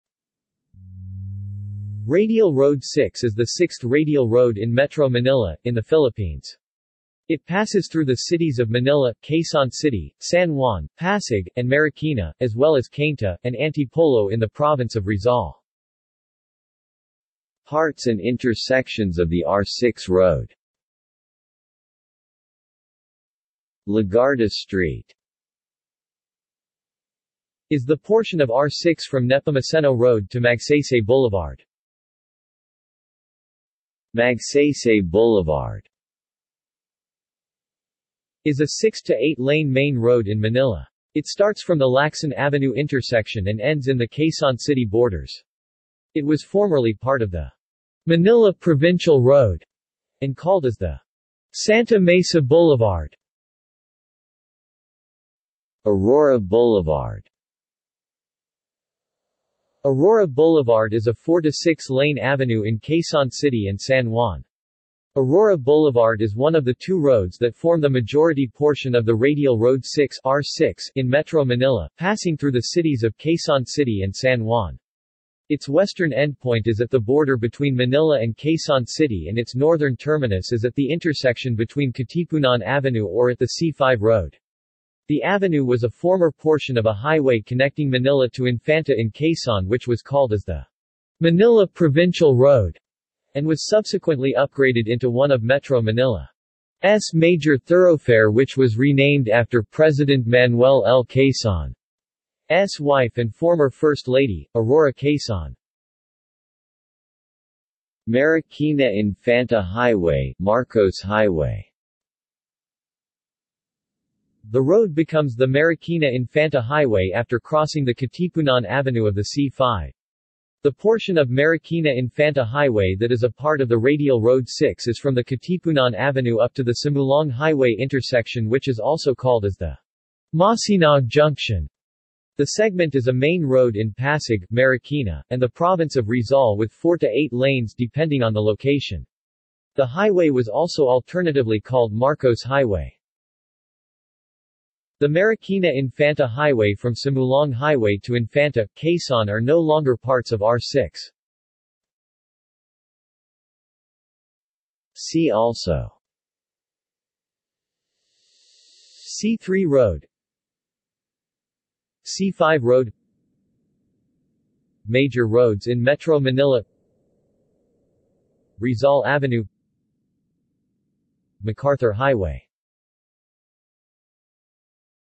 Radial Road 6 | Wikipedia audio article
This is an audio version of the Wikipedia Article:
Radial Road 6

SUMMARY
Radial Road 6 is the sixth radial road in Metro Manila, in the Philippines. It passes through the cities of Manila, Quezon City, San Juan, Pasig, and Marikina, as well as Cainta, and Antipolo in the province of Rizal.